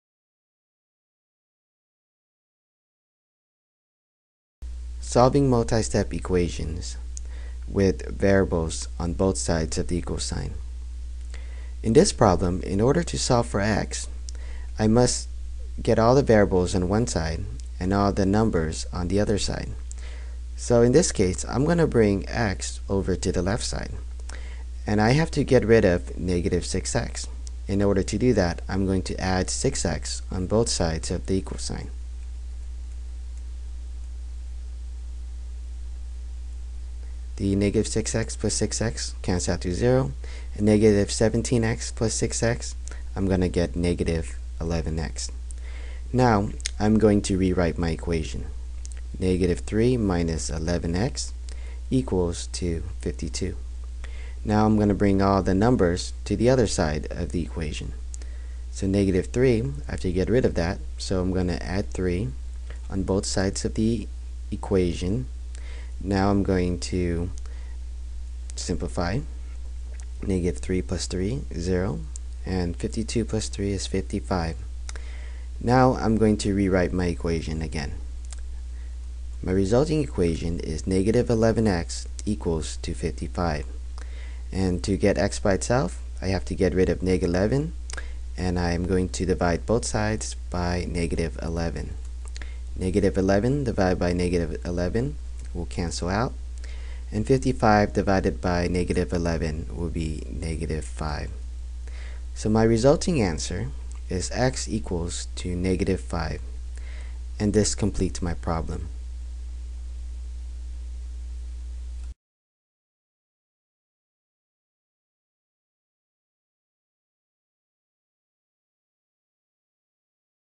Solving Multi-Step Equations with Variables on Both Sides
- How to Solve Multi-Step Equations with Variables on Both Sides .  For more practice and to create math worksheets, visit Davitily Math Problem Generator at